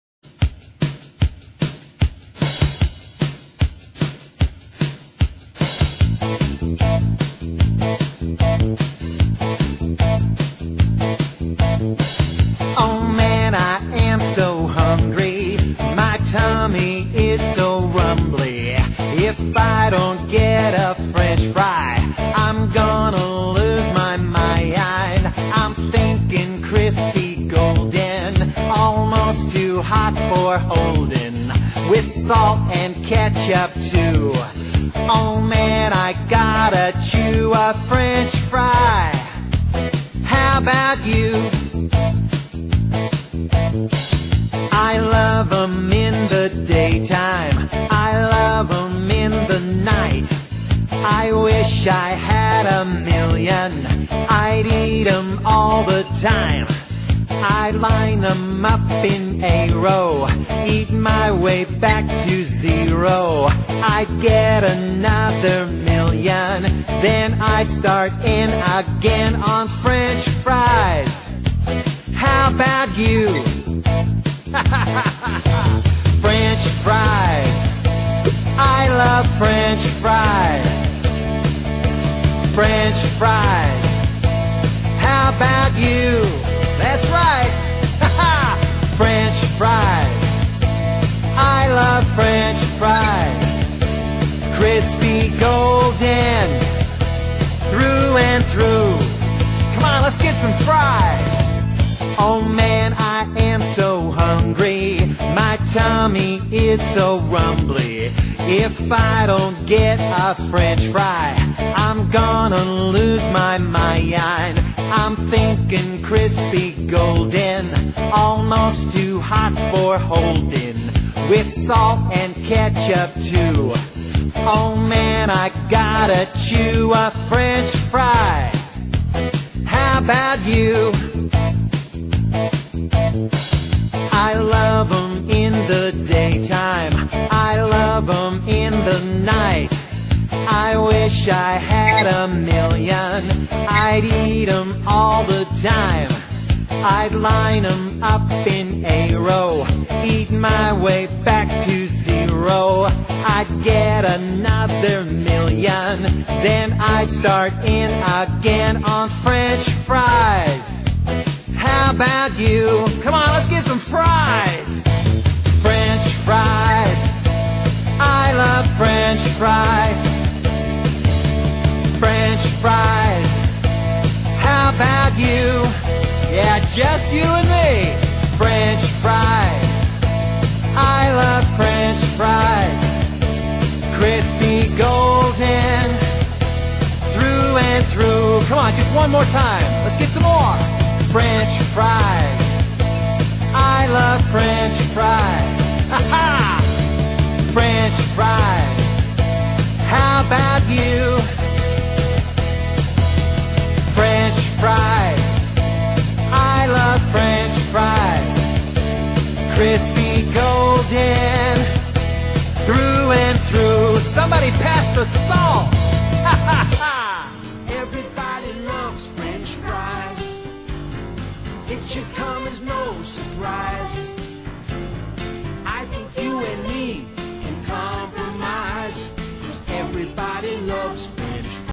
If you cant afford mspaint then Mcdonalds will do
Hear is what to do if you are not amused with your food at Mcdonalds or if you just want to pass the time whilst they nuke you a burger